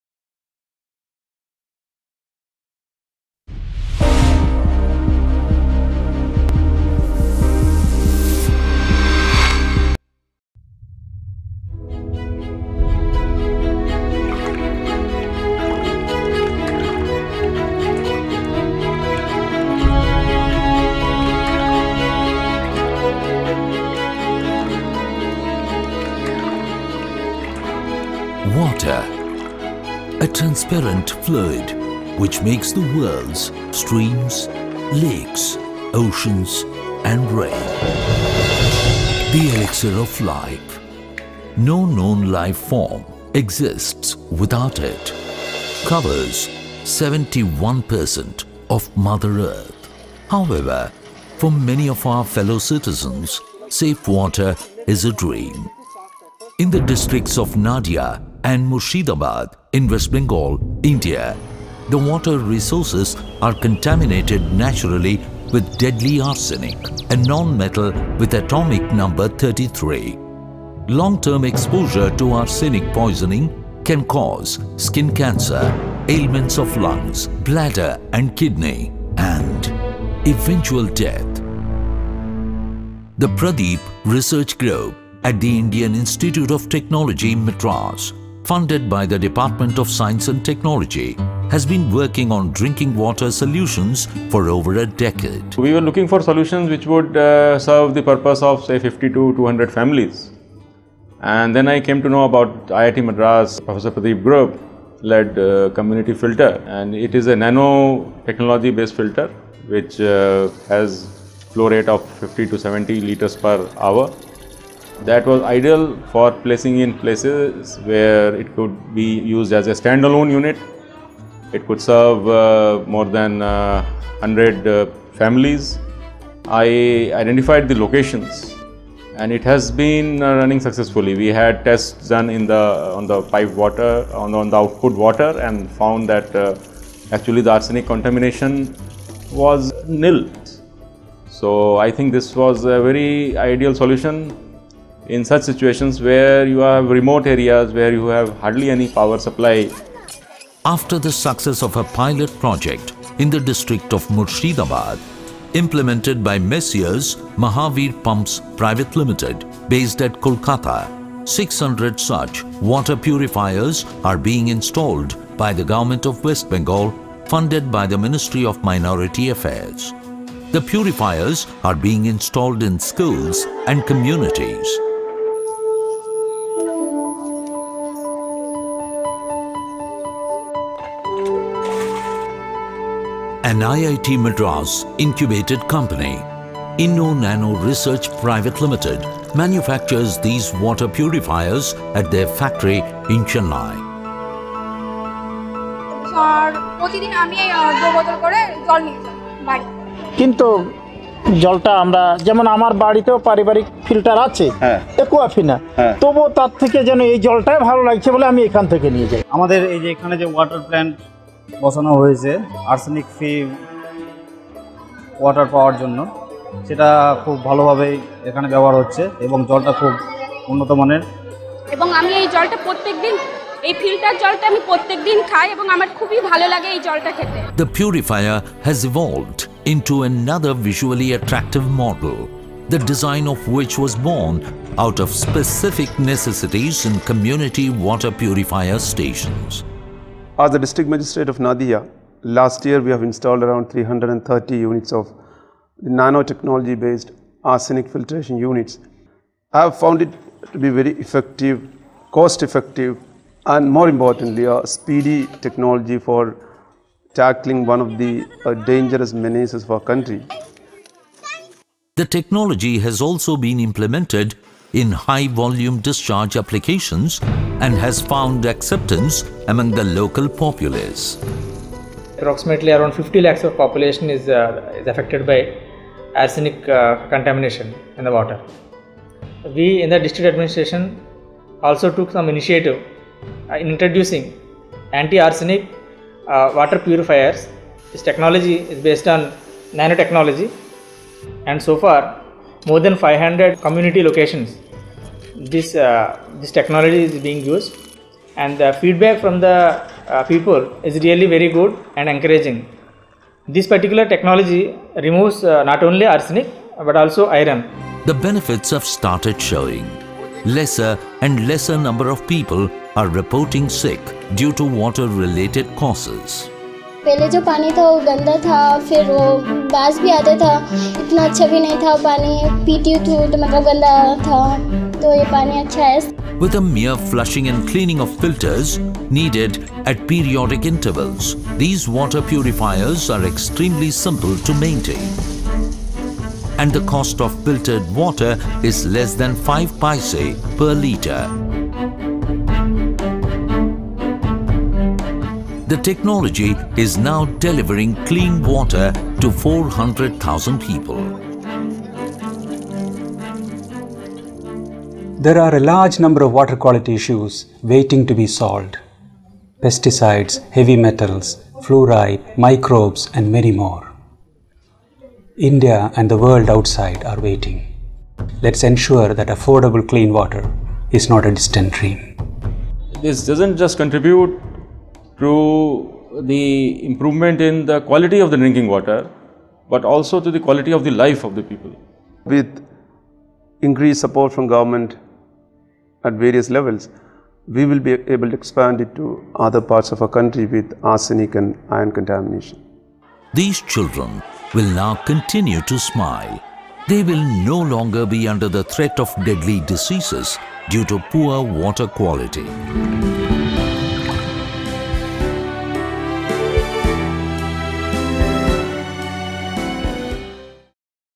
Arsenic free drinking water
The nanoparticle-based water filter developed in Prof. Pradeep’s lab requires no electricity to run, is adaptable to local conditions – the variations in the quality of water and the nature of its contaminants – can be scaled-up from meeting the requirements of a household to that of an entire community, needs little or no maintenance, and is environment-friendly. And affordable.  The aim behind the purifier was to provide safe drinking water without any microbial and chemical impurities, and to fight the problem of arsenic contamination that leads to many health hazards.